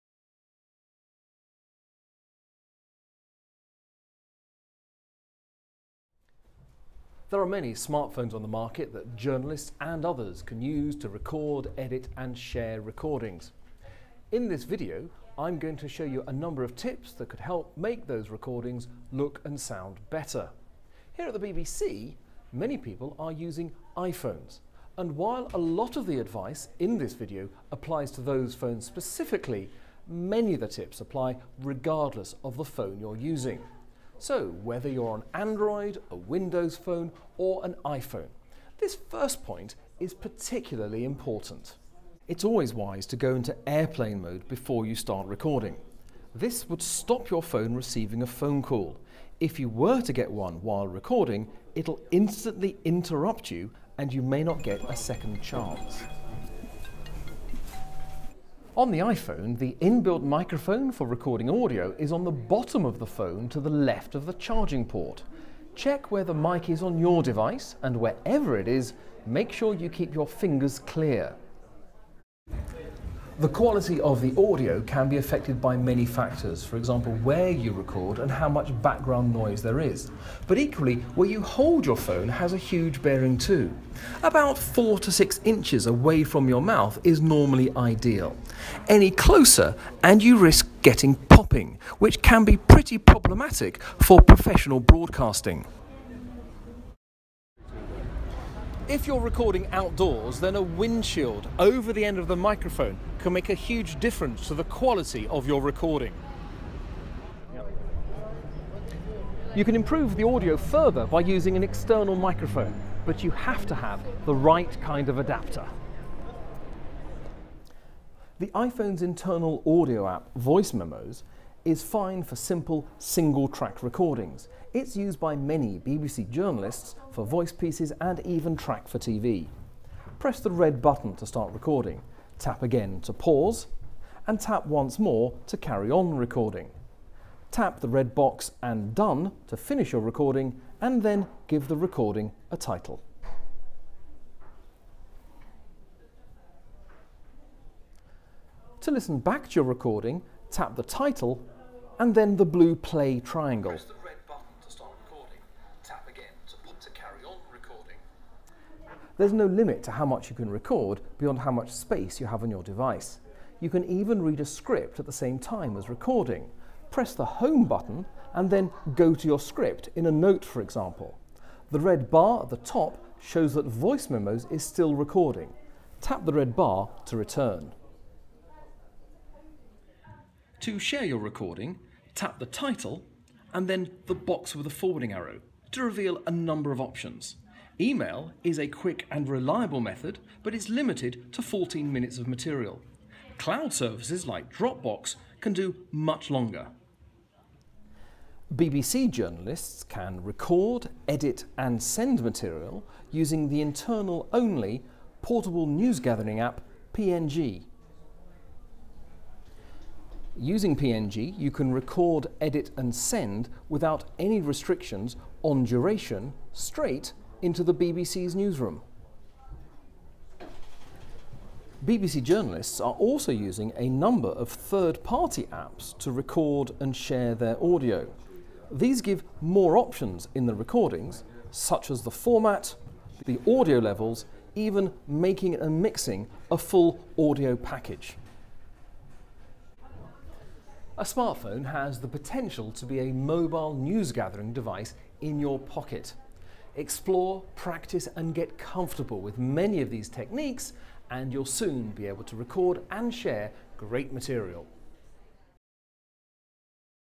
Smartphone journalism: Audio
Smartphones offer a simple and flexible way to record and edit audio. Our expert Marc Settle looks at how to use iPhones running iOS 9, or other mobiles, to create and edit broadcast-standard sound.